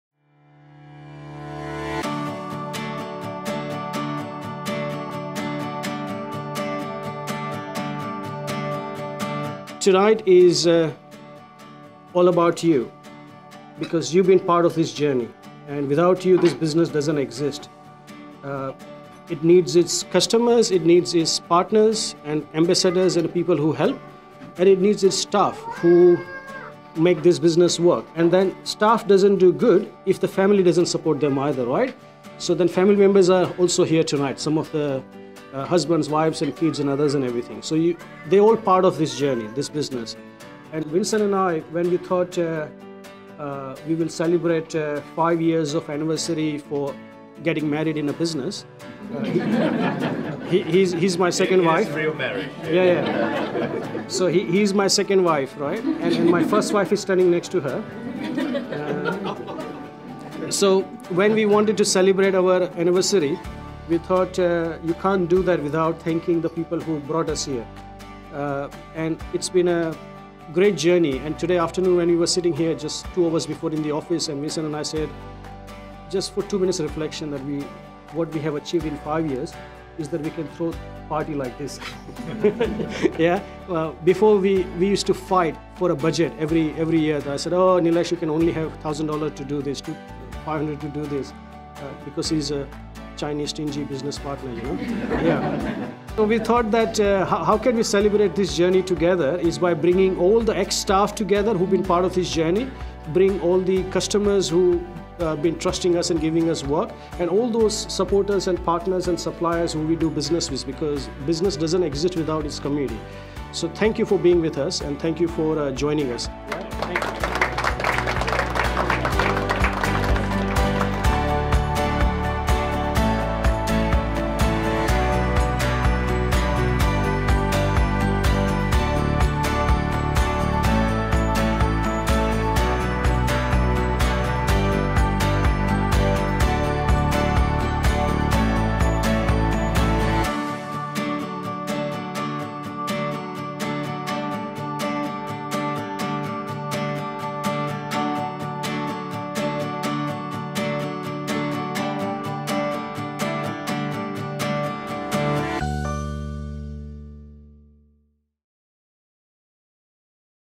illuminance Solutions celebrates 5 years
On Thursday 3rd December 2020 we invited everyone that has been a part of our journey since 2015 for a celebration of our success, and to say thank you. Here's looking back on some of illuminance Solutions' highlights!